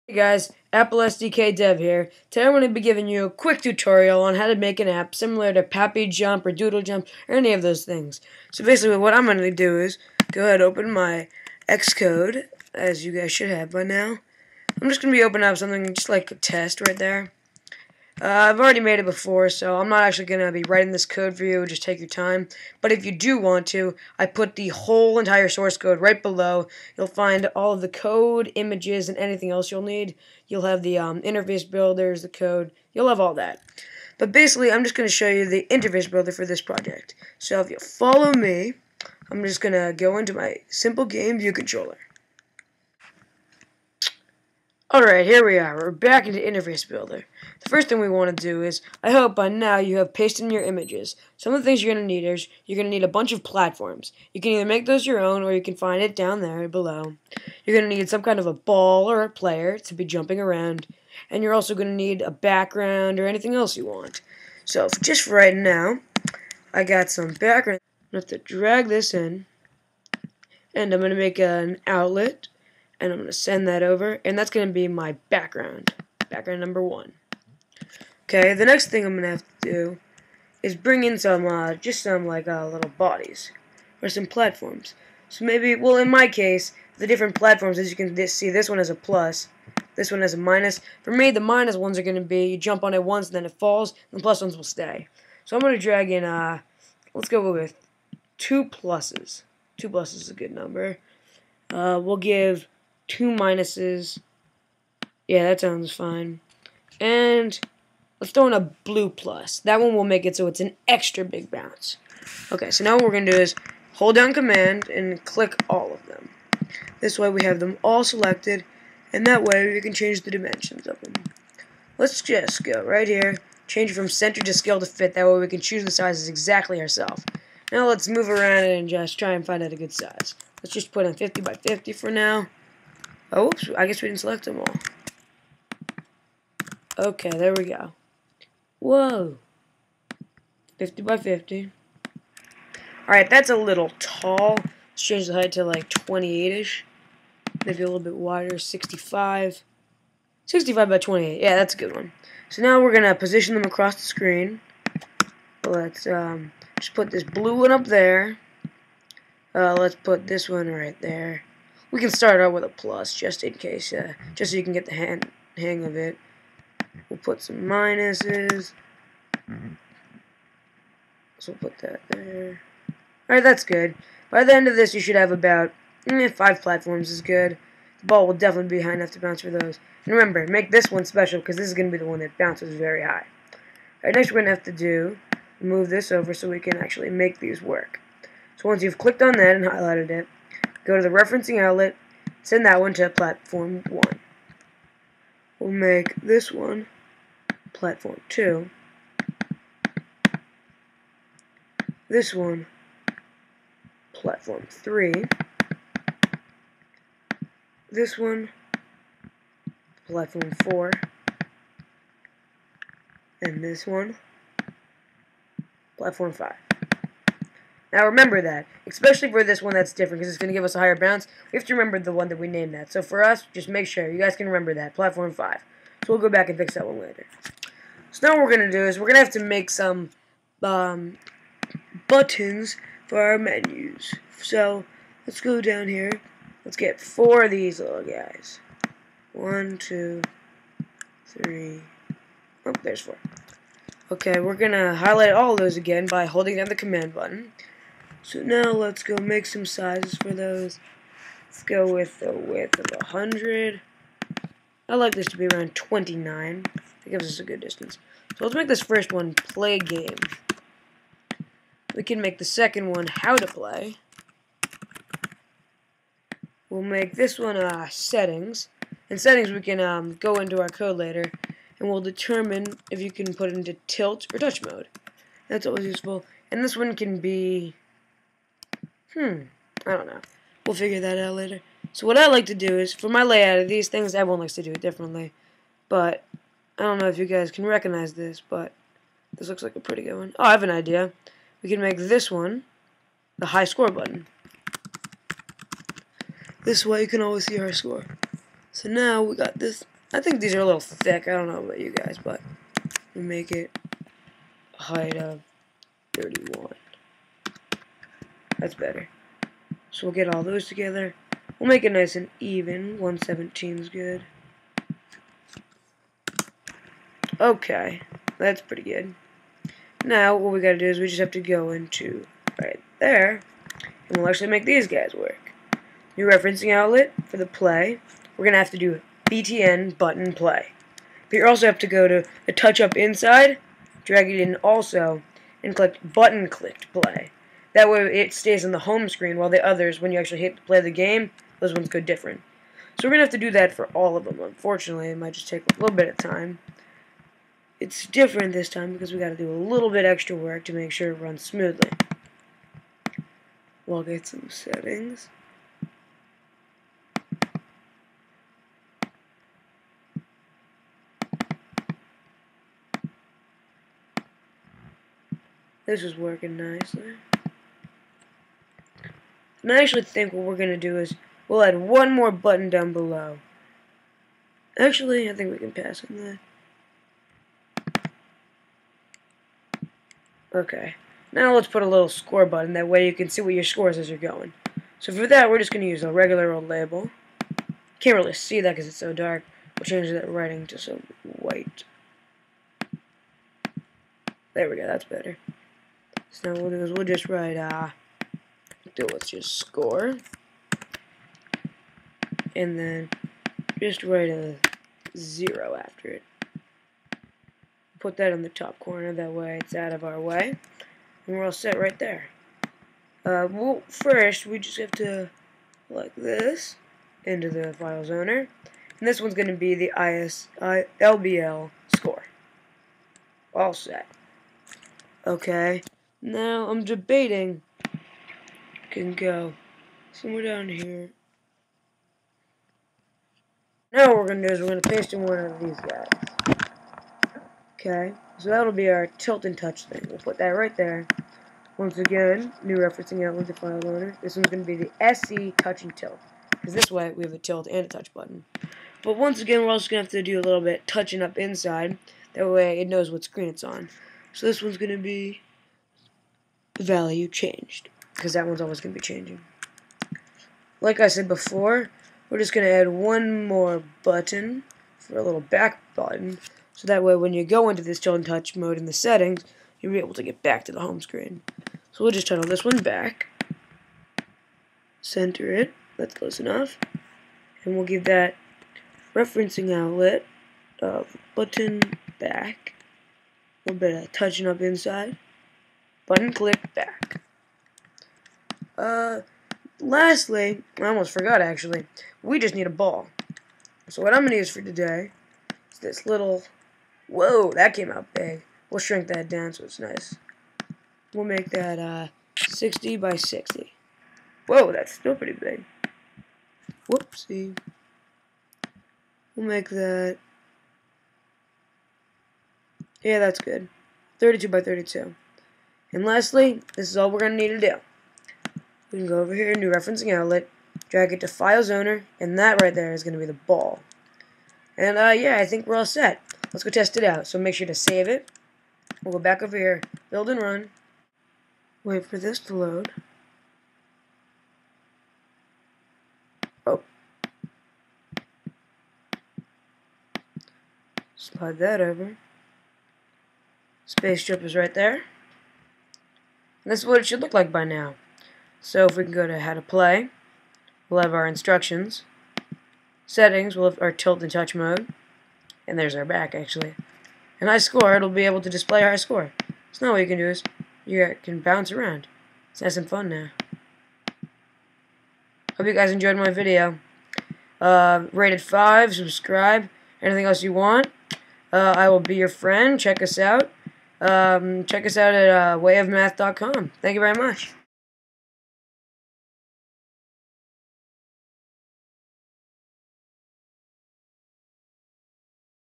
How To Make PapiJump: Xcode
Space jump is an app similar to PapiJump and DoodleJump!
IOS Developer
Video shows interface builder and simulator.

(credit to eagle1100)